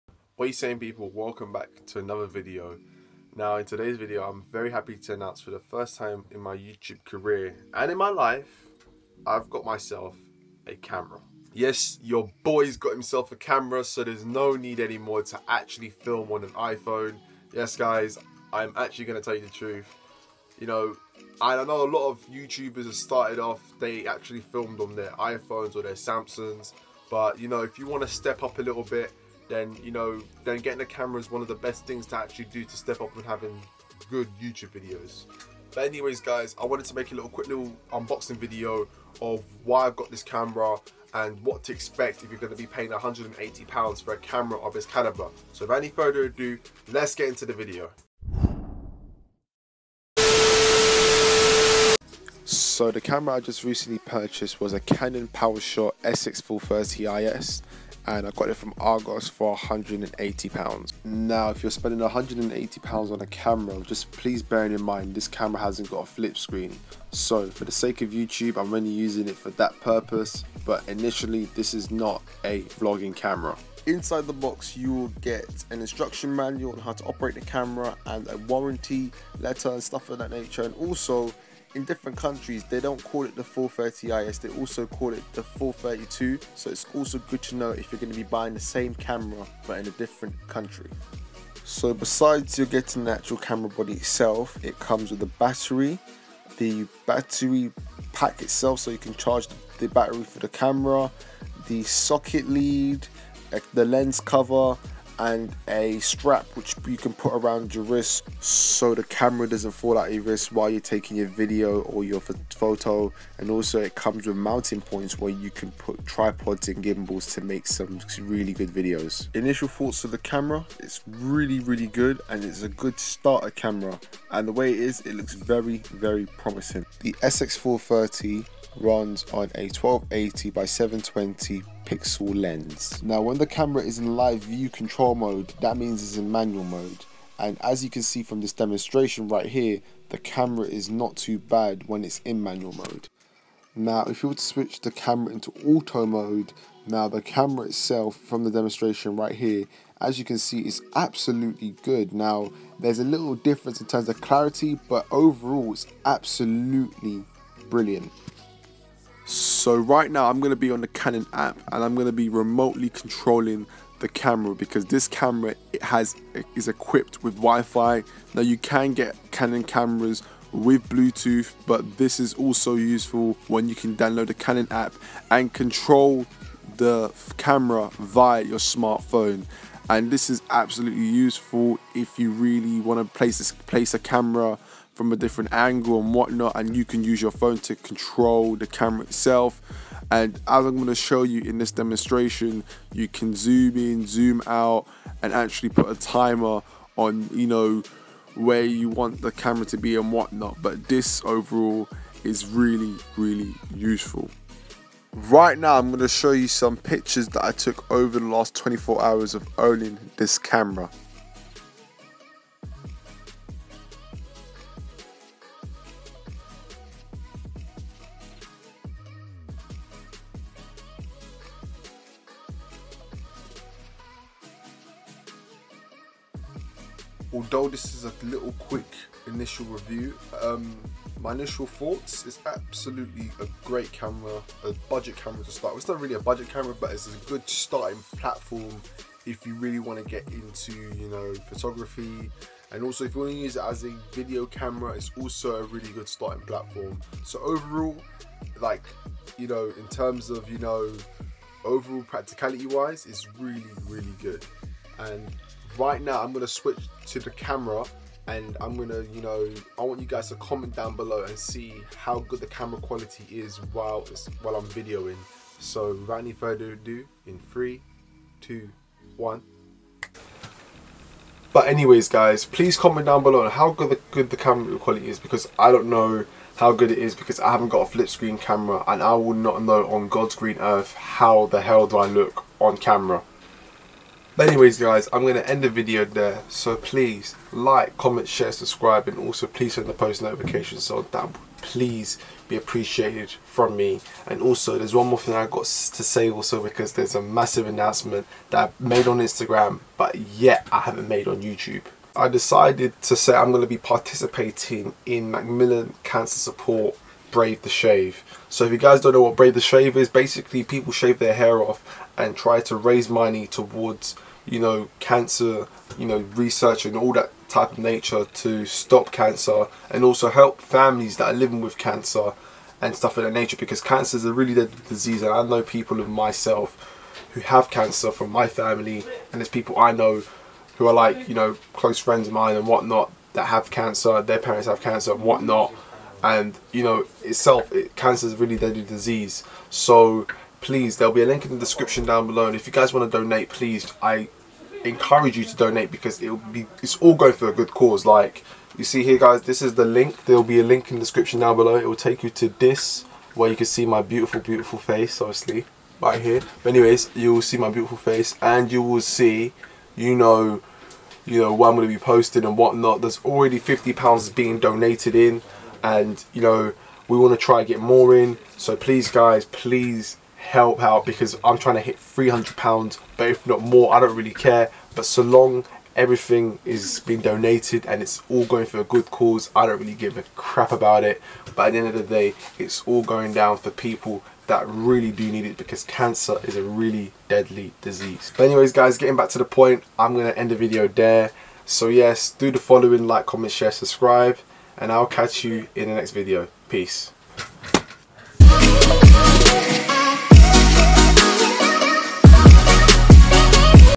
Unboxing My new camera *CANON* Powershot SX430 IS
*This is an unboxing video*, still very amazed that this camera only cost £180 from Argos and still have a lot of potential but I will make another video on this and do a video on the pros and cons of this camera in the near future.